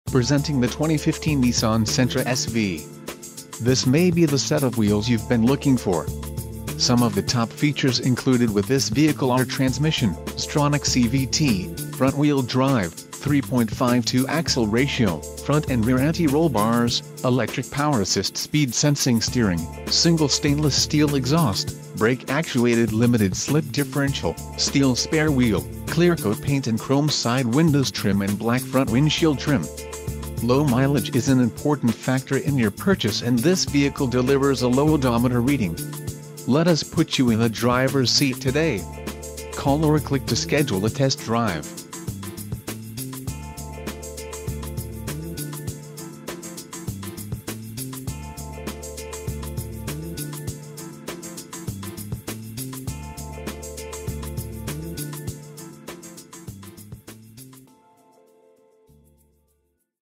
2015 Nissan Sentra SV - Kelly Car - Emmaus, PA 18049
2015 Nissan Sentra SV Condition: New Mileage: 6 Transmission: CVT with Xtronic Exterior Color: Gun Metallic Interior Color: Charcoal Doors: 4 Stock: N52124 Cylinders: 4